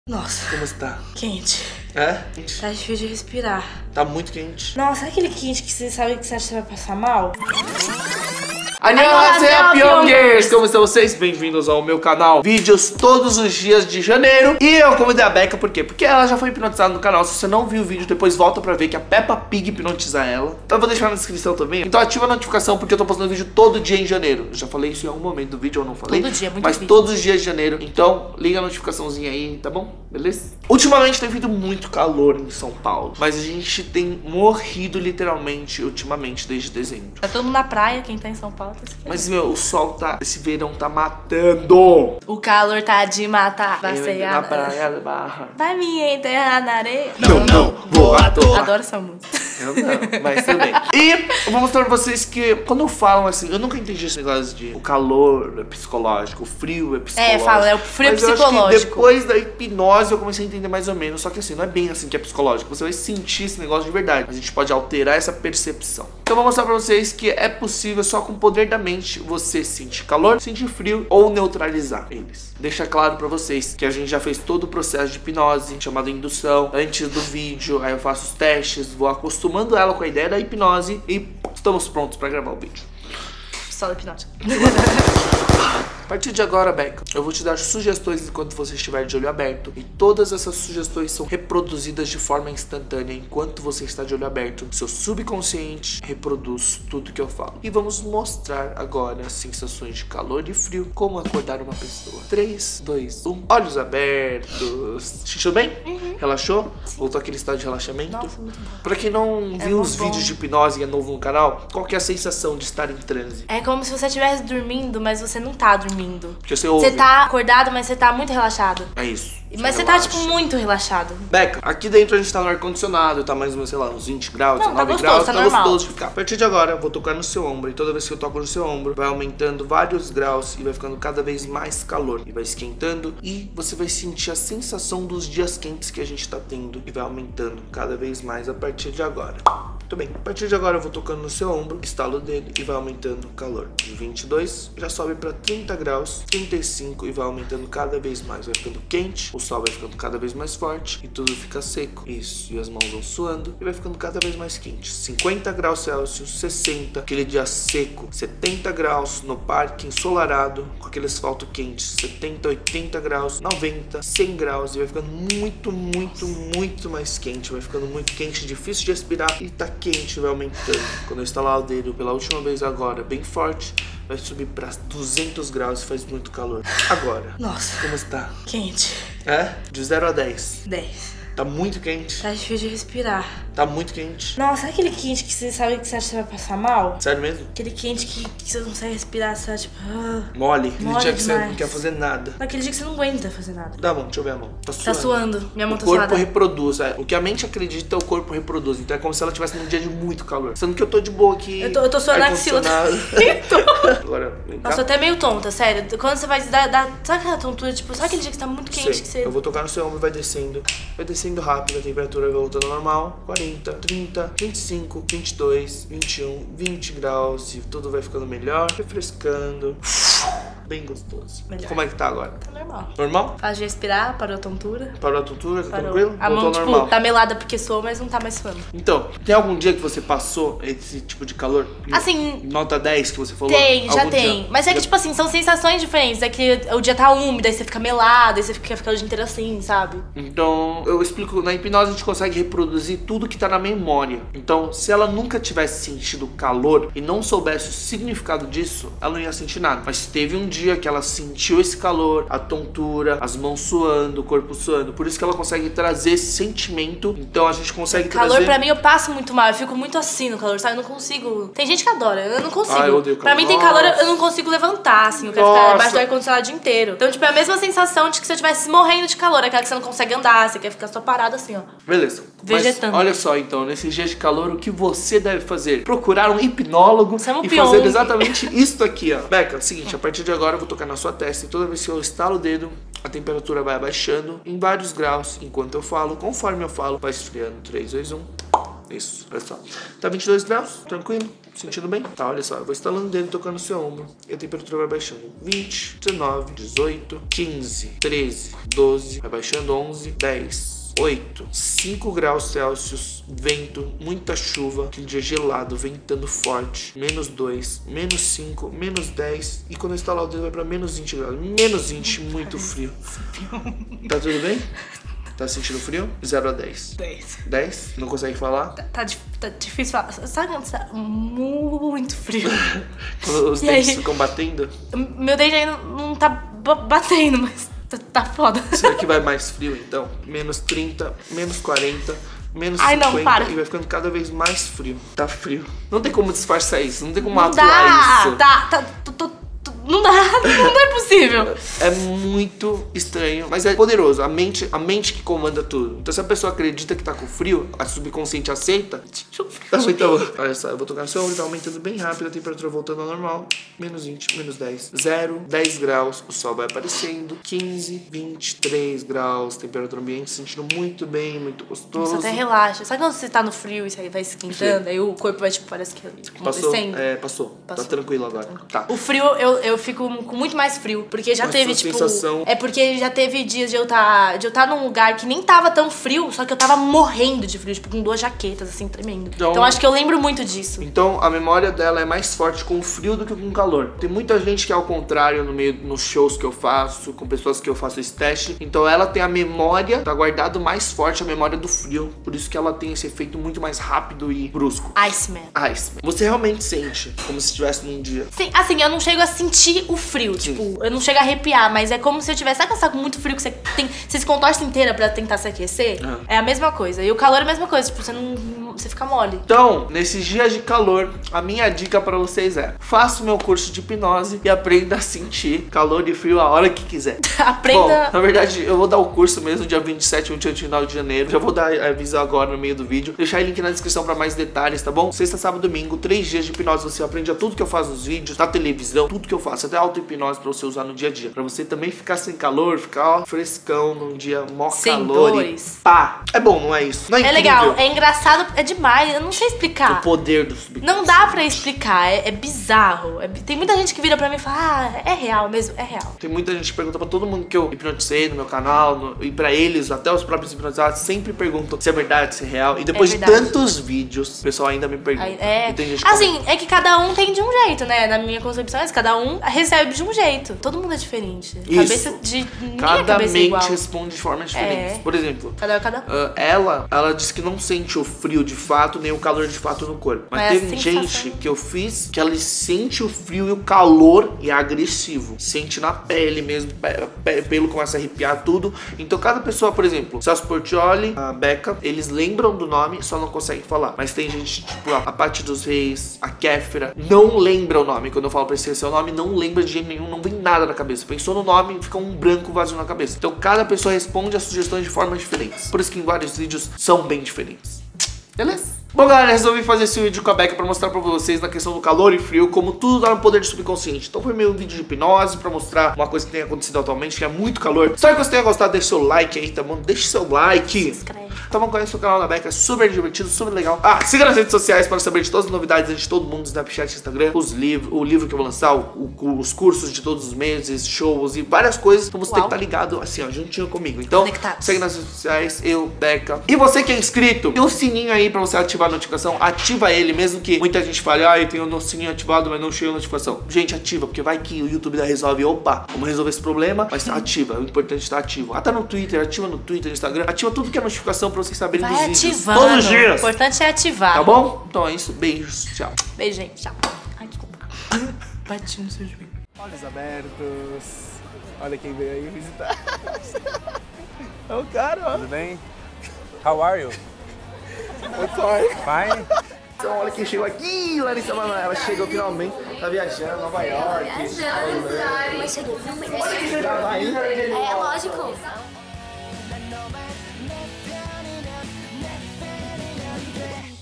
234/ HIPNOSE : CALOR DE PASSAR MAL ! (com Becca Pires )
★ Aqui em SP o calor está insuportável. Será que com hipnose podemos resolver isso? Convidei a Becca Pires pra a gente fazer uma demonstração. Não esquece de compartilhar se curtir!

► pyonglee

Fotografia e edição: Daniel Docko